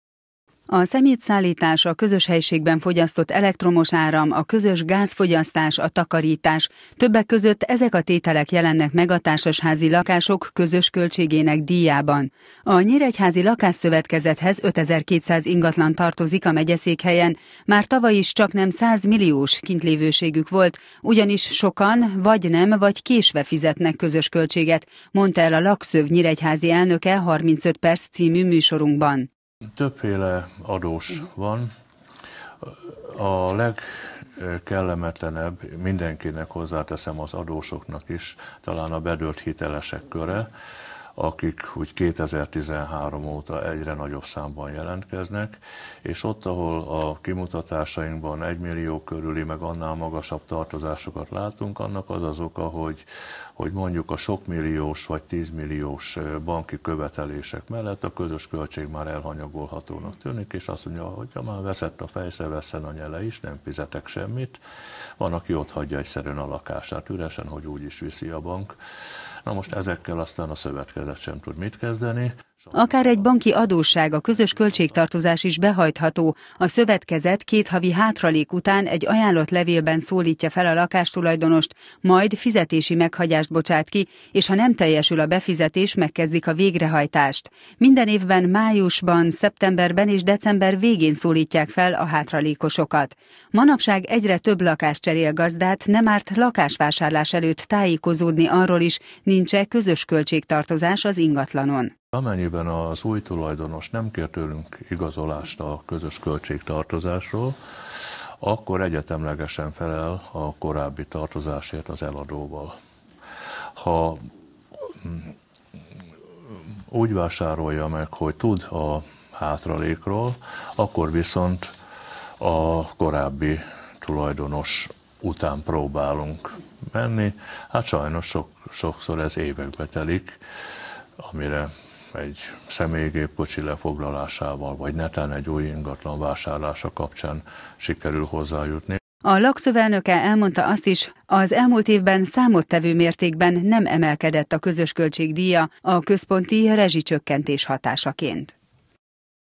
Sok a kintlévőség - Kölcsey Tv
Évente mintegy 10 százalékkal növekszik azoknak a háztartásoknak a száma, ahol vagy nem fizetnek közös költséget, vagy késnek a díj teljesítésével- mondta el a Nyíregyházi Lakásszövetkezet elnöke televíziónknak.
A probléma nemcsak a megyeszékhelyen tapasztalható hanem országos szinten is tetemes adósság terheli a társasházi és lakásszövetkezeti otthonok több mint négy és fél millió lakóját. A bedőlt devizahitelesek után nem ritka, hogy a közös költség tartozást szinte lehetetlen behajtani.